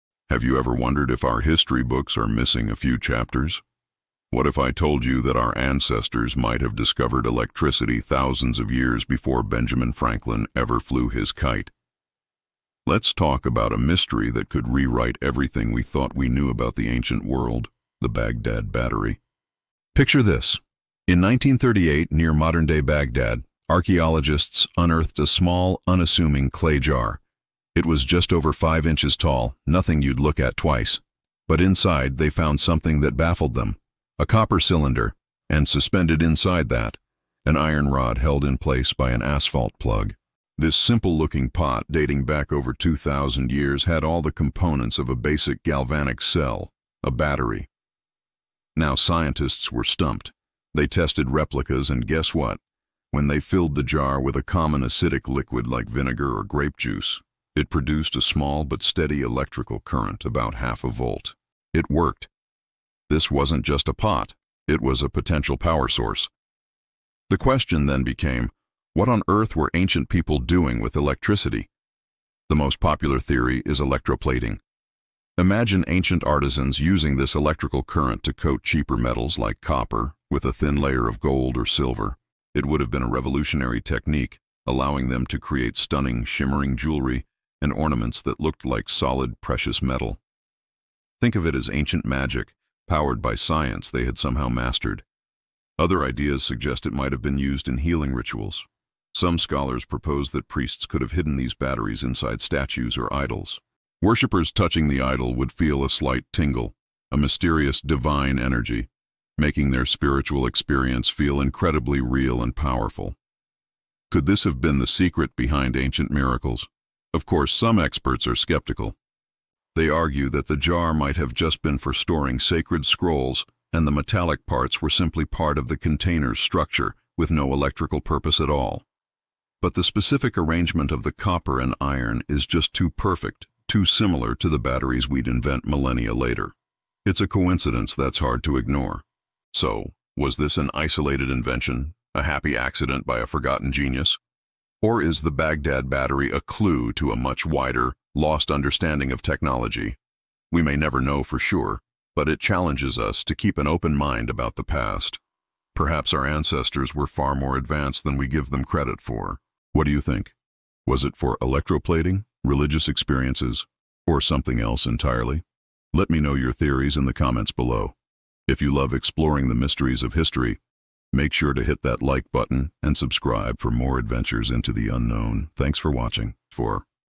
The Lost Civilization That Mastered Electricity
📌 The Baghdad Battery — an artifact thousands of years old — may prove ancient people harnessed electricity long before modern science.